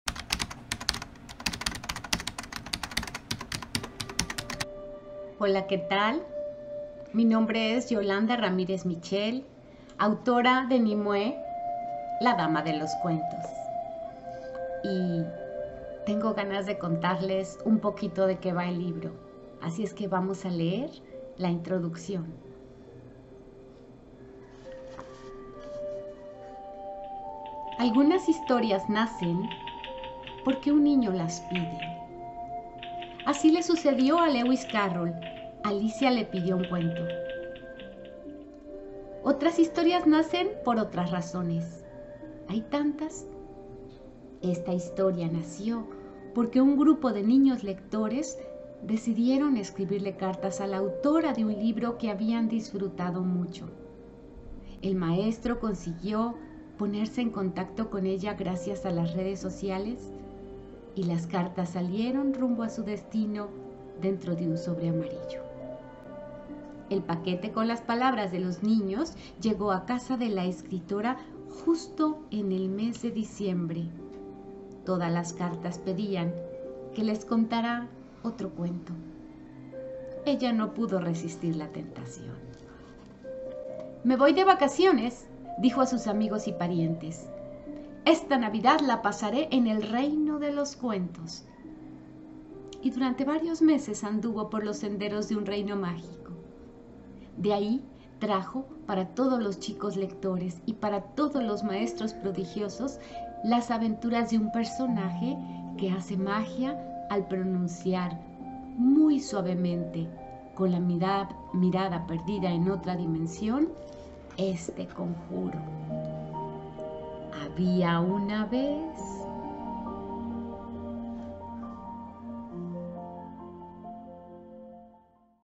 Introducción: "Nimué la dama de los cuentos".
Esta es la introducción del libro, Nimué, la dama de los cuentos. Publicado por la editorial Panamericana.